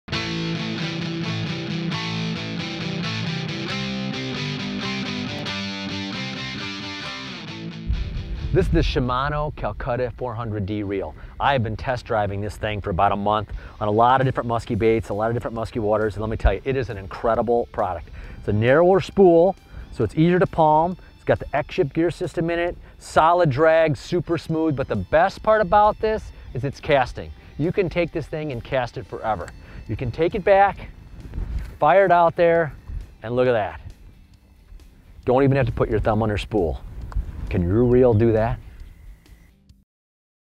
Shimano Calcutta 400D Reel
Jim Saric discusses the advantages of the Shimano Calcutta 400D.